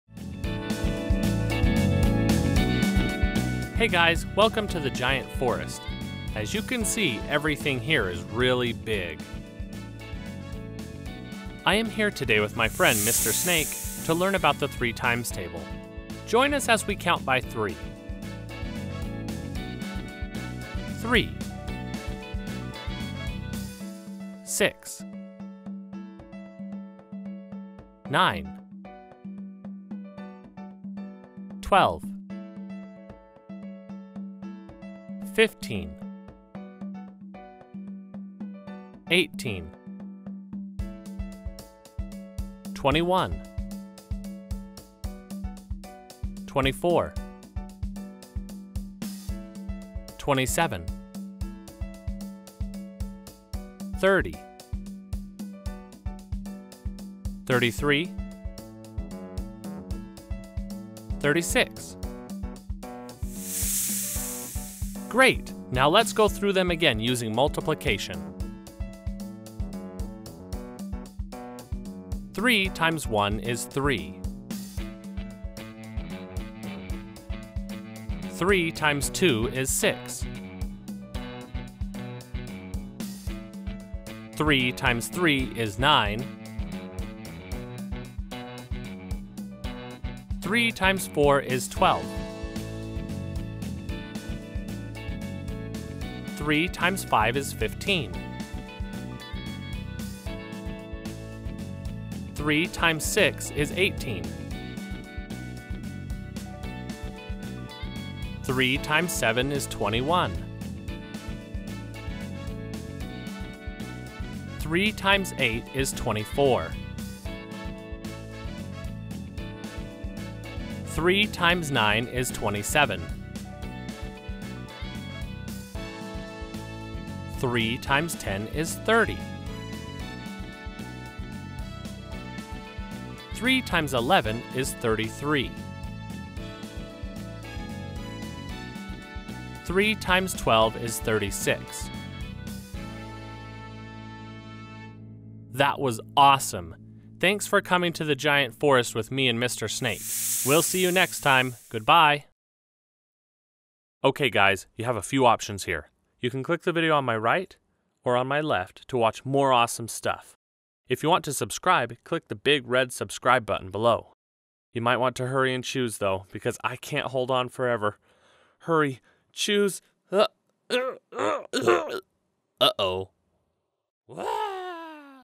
Three Times Table Video with Mr. Snake - Multiplication Math Videos for Kids
Slither along with our new friend Mr. Snake as we cover the three times tables. We first count by 3 all the way to 36 and then learn the multiplication for the three times table.

Learning to memorize the times tables in 2nd and 3rd grade are very important to a child's ability to learn other important math concepts. Our fun math videos help kids memorize and learn important math concepts in the lower grades.

In mathematics kids learn to multiply as early as 1st grade but for most memorizing the times tables starts around 3rd grade.